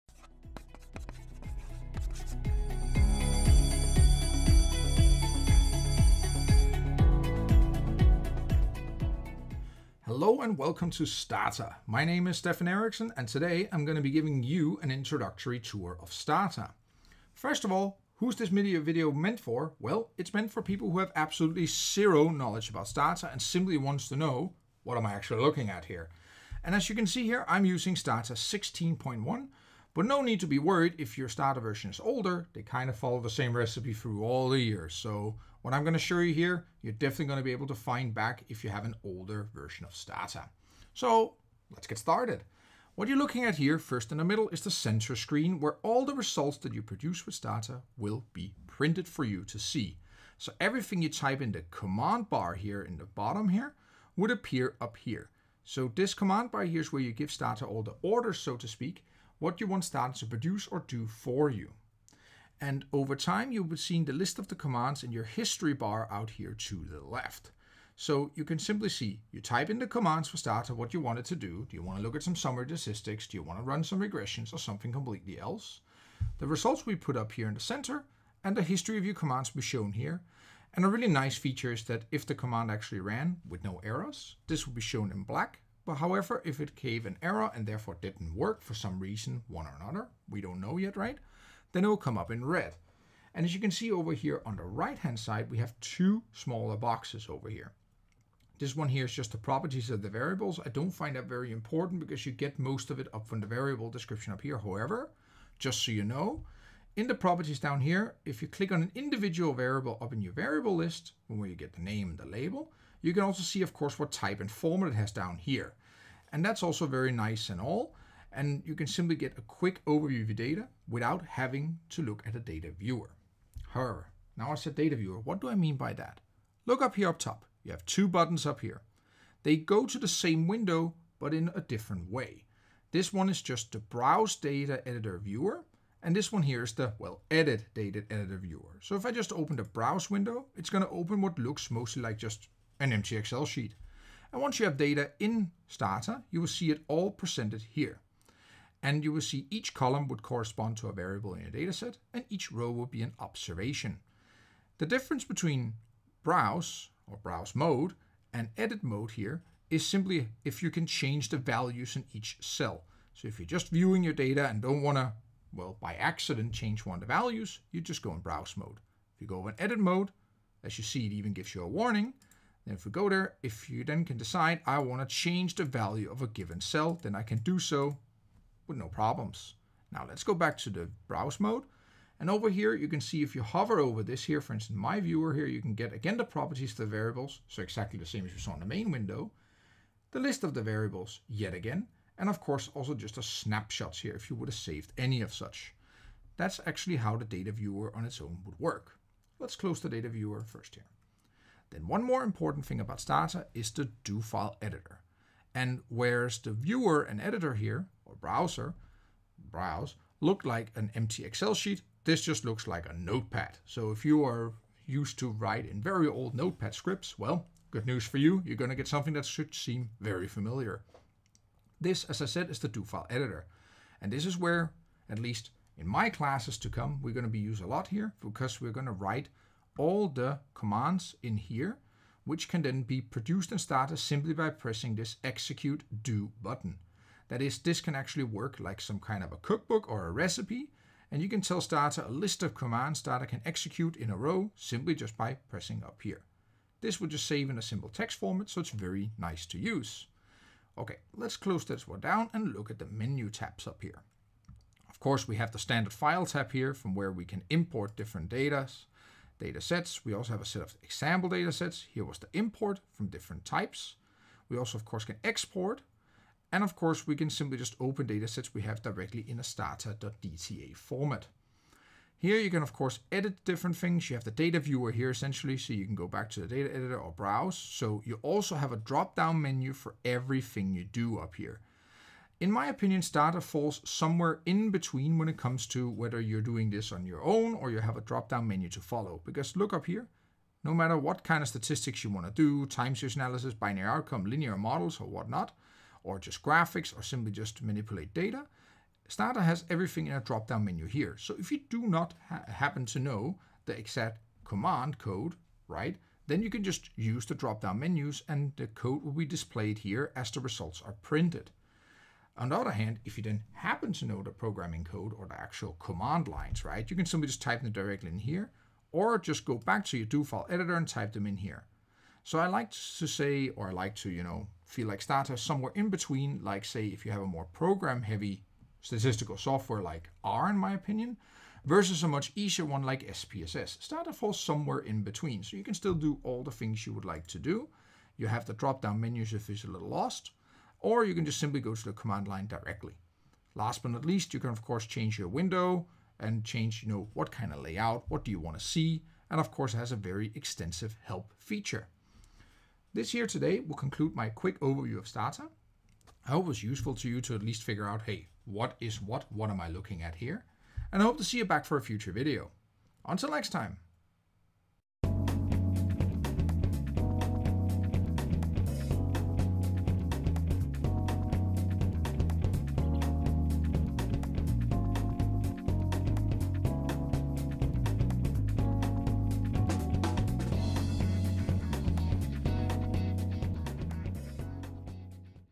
Stata - A Quick Introduction
Welcome to my classroom!
This video is part of my Stata series. A series where I help you learn how to use Stata. In this video, I give an introductory tour!

Note: What I show here is my take on the topic. I would be happy to receive comments!